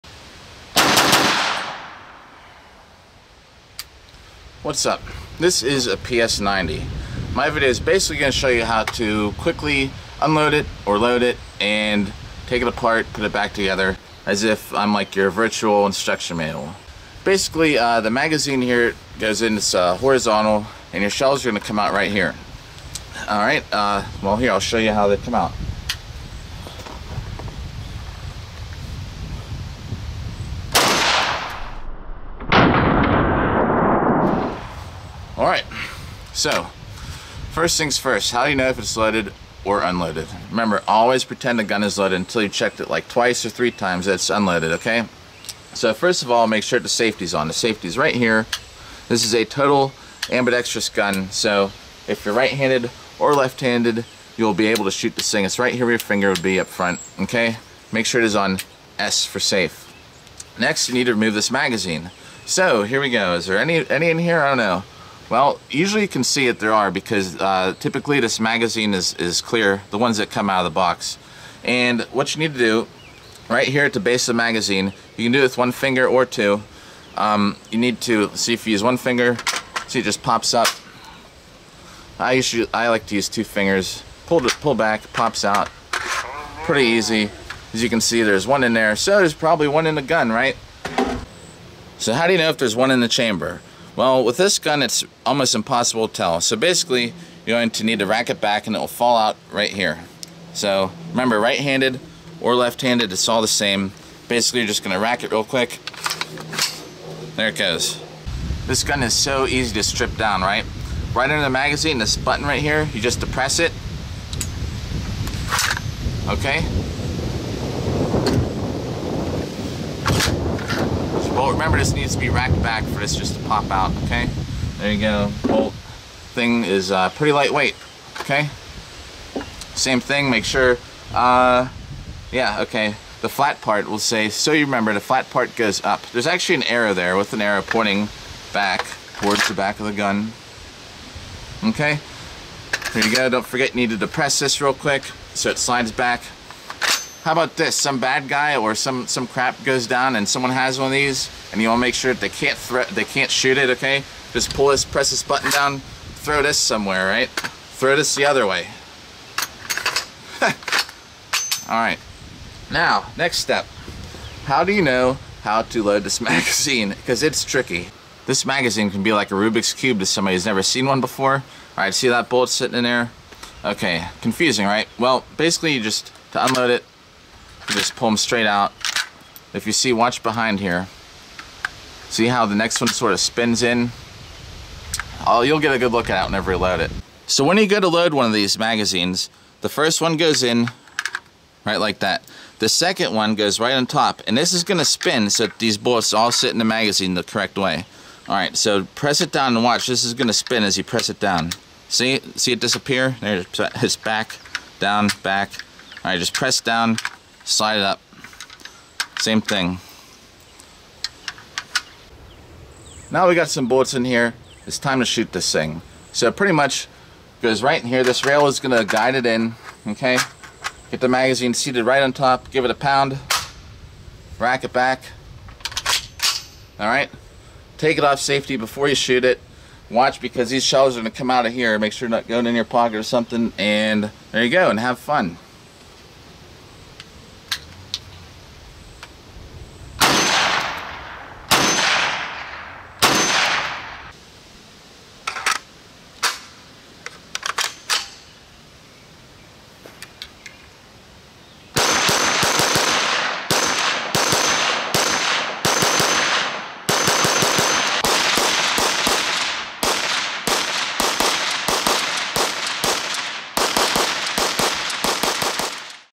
PS-90 (How to: Unload, Strip, Load, Shoot)
Quick video guide/tutorial to the FN PS-90. How to unload, field strip, load (those wacky 50 round magazines), and then shoot.

Recorded this at Iron City Armory, in Pittsburgh, Pennsylvania where I am a member. If you are in town, I highly recommend them for the best indoor and outdoor shooting ranges.